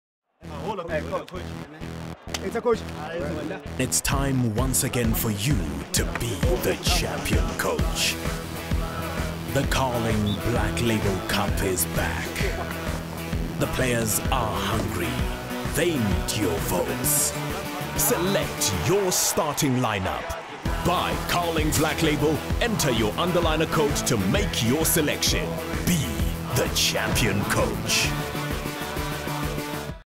Carling Black Label - Coach / Sunu (Egg Films)
Egg Films' Sunu directed Carling Black Label's Coach commercial.